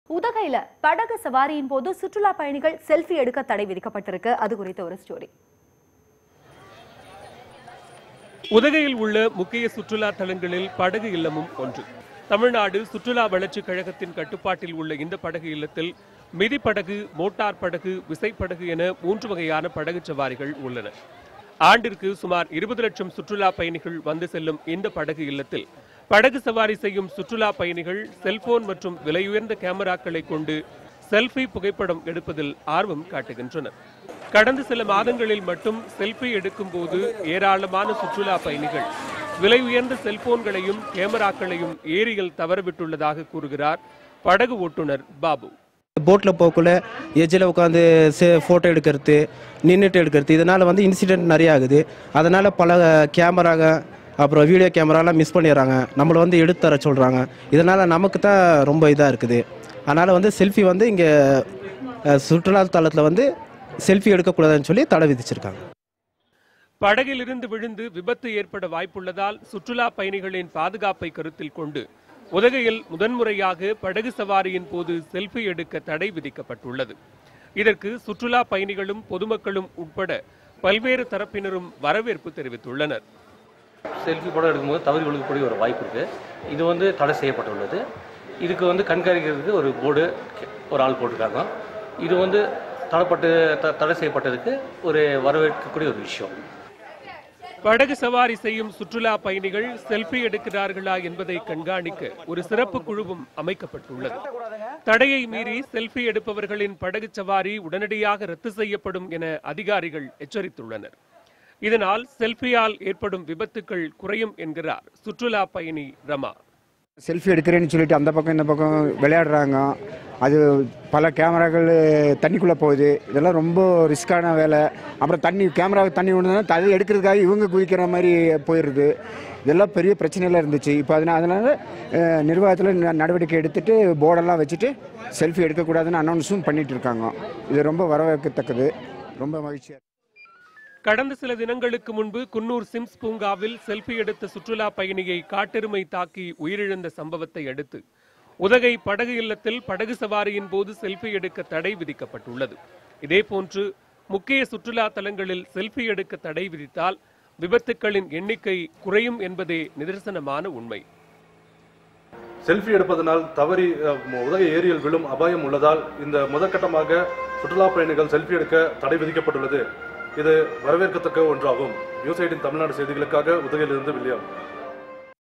செல்பி தடைக்கு வரவேற்பு - உதகை | Reporter 18 | News18Tamilnadu
உதகை ஏரியில் படகு சவாரியின் போது சுற்றுலா பயணிகள் செல்பி  எடுக்க தடை விதிக்கபட்டுள்ளது. இது குறித்து விளக்குகிறது இந்த செய்தித்தொகுப்பு